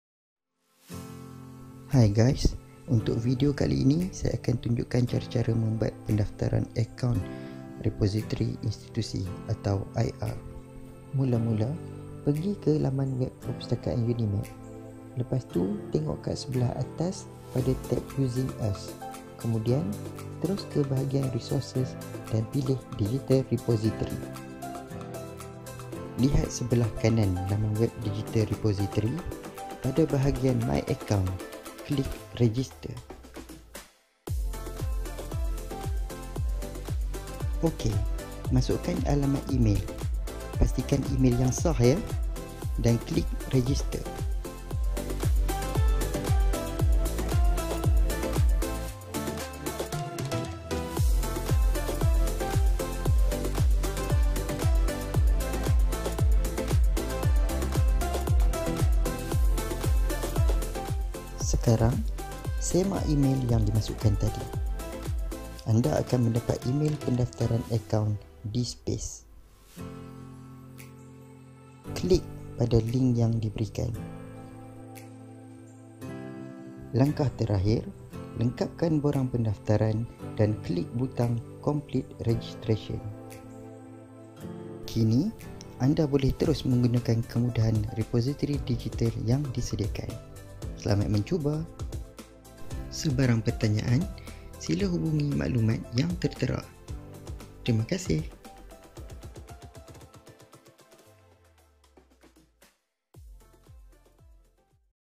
Pendaftaran Repositori Institusi(IR)@PTSFP
DSpace is built to store, share and search UniMAP's research and scholarly materials. UniMAP Library Digital Repository provides access to thousands of scholarly materials produced by UniMAP community. You will also find other relevant materials such as full text content of theses, past year exam papers, selected government circulars, speeches, reports, and many others. Contribute now and make your publications accessible online!.

Items in UniMAP Library Digital Repository are fully indexed and searchable on Google Scholar.